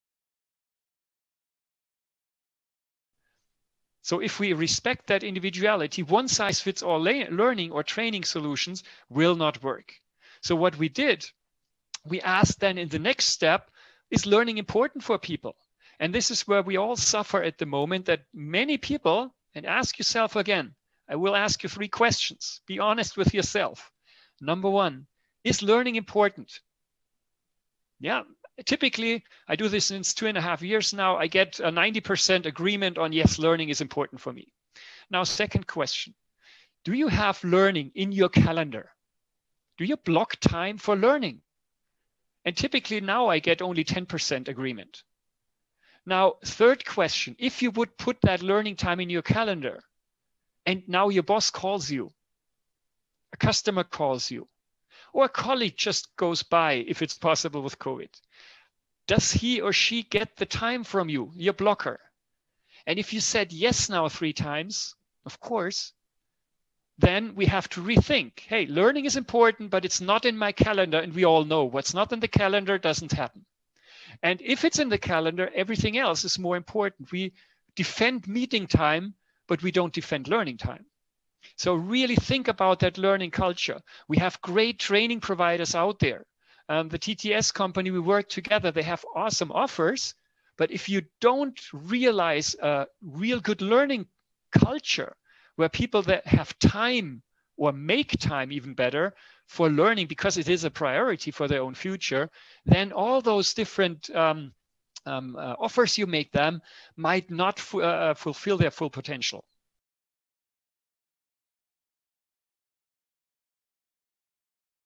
What priority does learning have in your work routine?
Harald Schirmer, Manager Digital Transformation and Change, Continental AG, "Look inside of digital resilience - Human side of transformation" at Business Strategies in Digital Transformation 2021